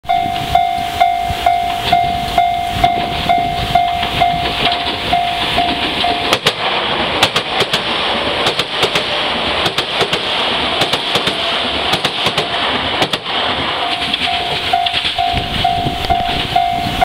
２０００系 （６両編成） 普通 福岡空港行き 【今宿駅前】
２０１１年０６月０２日に撮影した（462C）です。